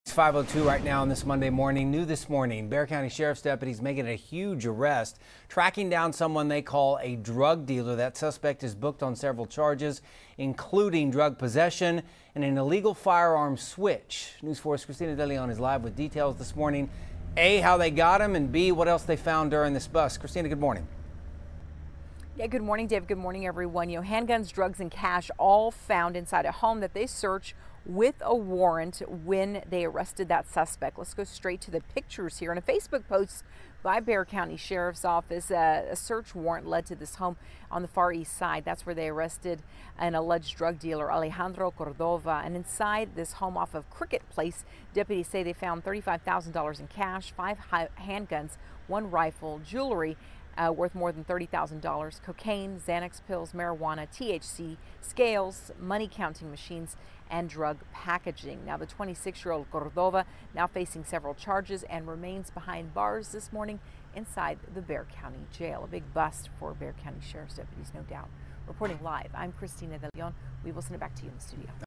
East Side Raid Leads to Cash, Guns and Plenty of Drugs
Raid of East Side home nets guns, drugs, and over $65,000 in cash and jewelry. A local man is now behind bars facing federal charges.